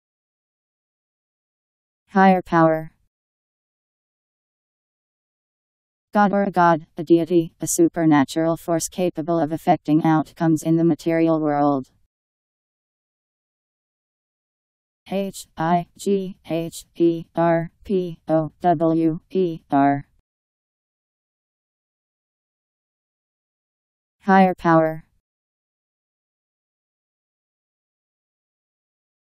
Higher power Meaning
Video shows what higher power means. God or a god, a deity, a supernatural force capable of affecting outcomes in the material world..  Higher power Meaning. How to pronounce, definition audio dictionary. How to say higher power. Powered by MaryTTS, Wiktionary